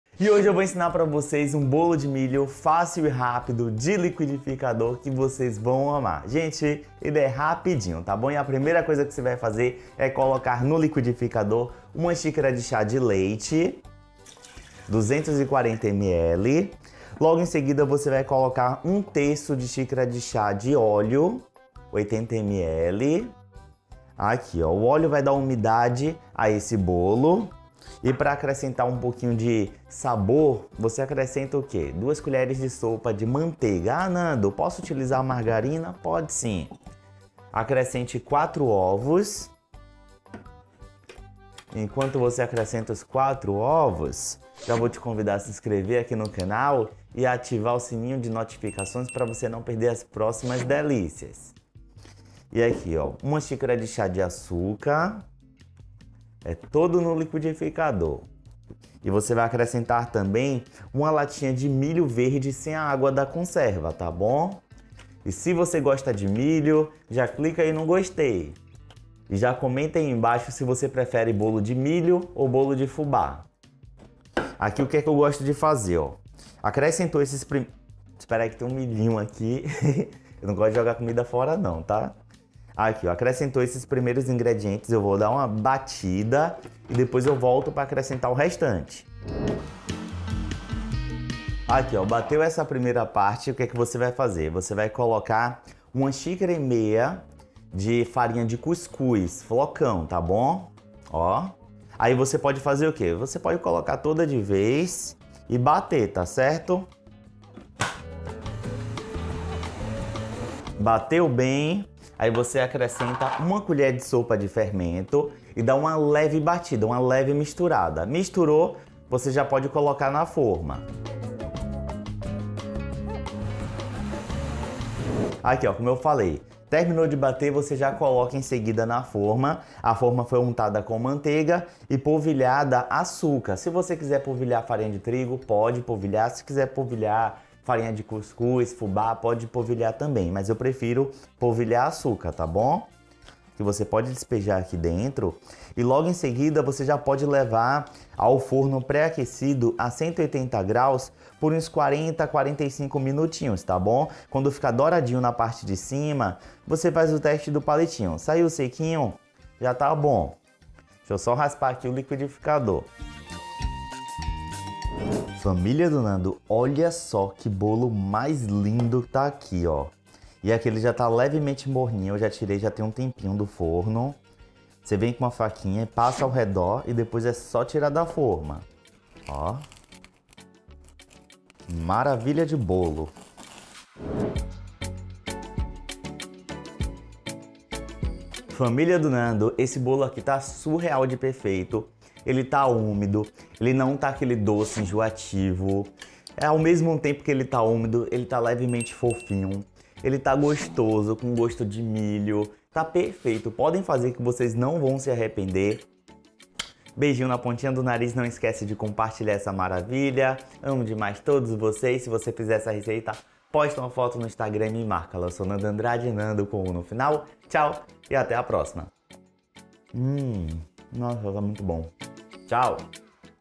BOLO DE MILHO FÁCIL E RÁPIDO DE LIQUIDIFICADOR | Nandu Andrade
A receita de hoje é um bolo de milho todo feito no liquidificador, bem fácil de fazer e muito gostoso! Vocês vão amar esse bolo!

BOLO DE MILHO FÁCIL E RÁPIDO DE LIQUIDIFICADOR

🔴 ingredientes:
1 lata de milho verde
1 xícara de chá de açúcar (200g)
4 ovos
⅓ de xícara de chá de óleo (80ml)
1½ xícara de chá de flocão
2 colheres de sopa de manteiga ou margarina
1 xícara de chá de leite (240ml)
1 colher de sopa de fermento

Tamanho da forma: 21x8cm